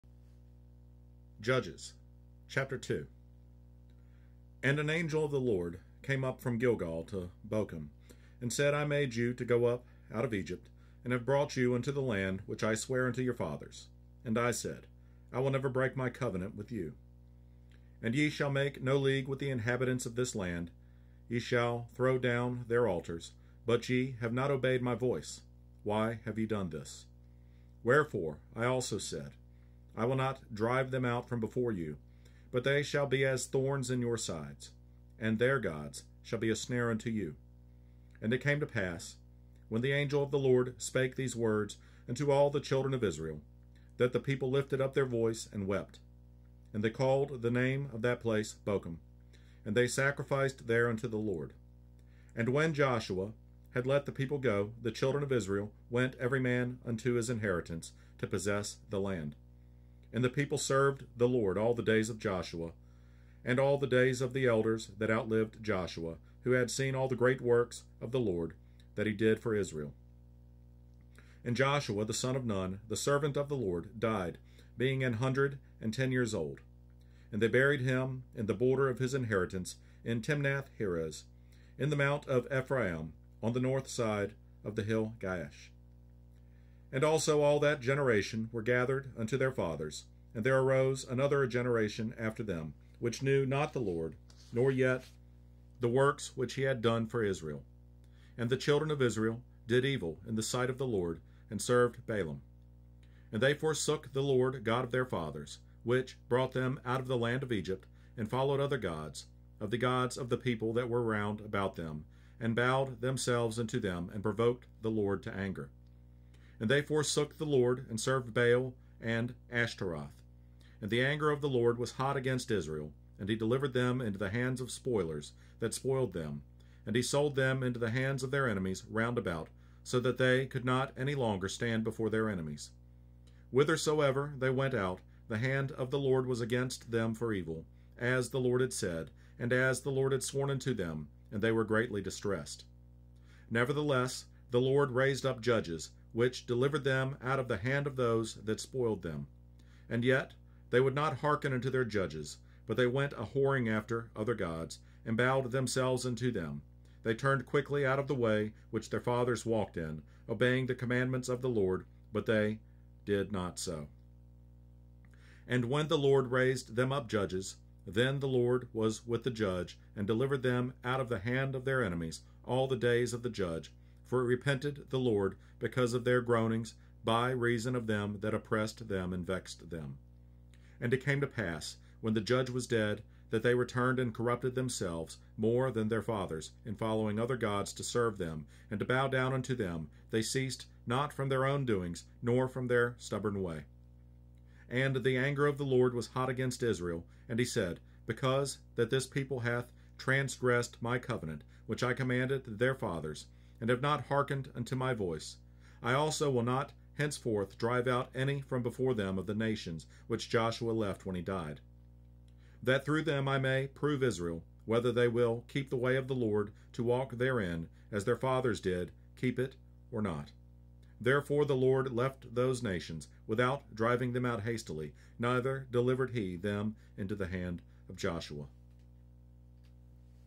Judges Chapter 2 (Holy Bible KJV)
1-23 (The Angel of the LORD at Bochim / The Death of Joshua / Israel's Apostasy and the Ministry of the Judges)